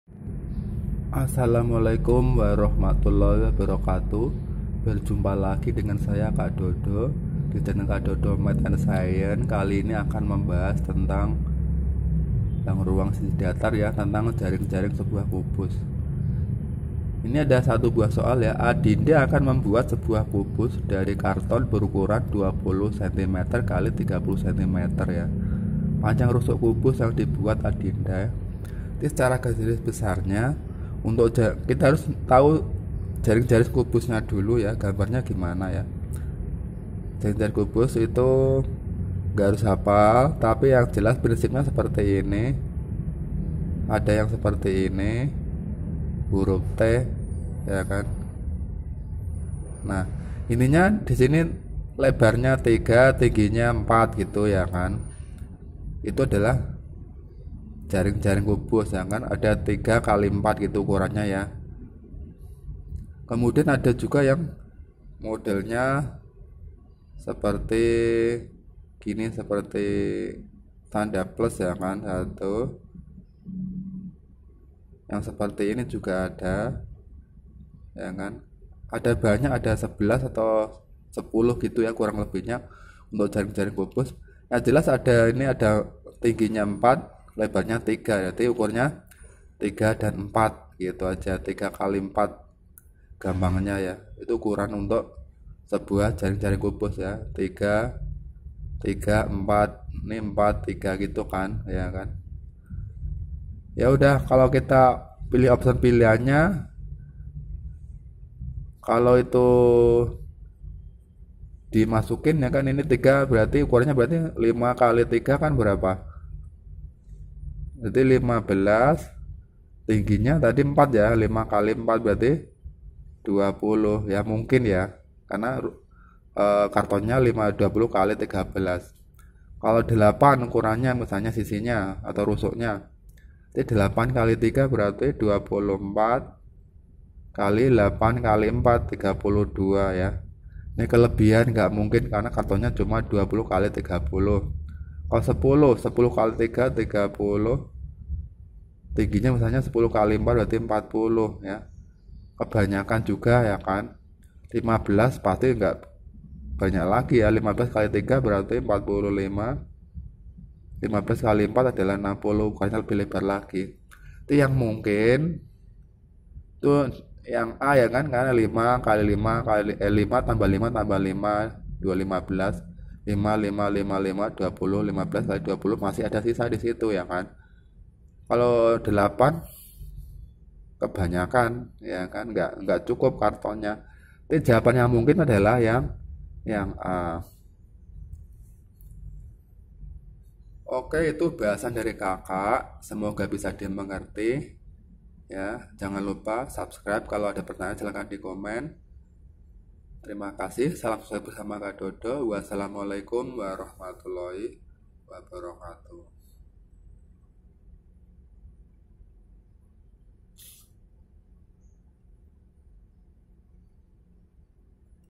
menghitung panjang rusuk kubus
menghitung panjang rusuk kubus berdasarkan luas karton yang tersedia semoga jadi bahan pembelajaran bagi siswa yang masih duduk dibangku SMP. jangan lupa subscribe like share dan komen demi perbaikan channel kakak. salam sukses bersama kak Dodo. terima kasih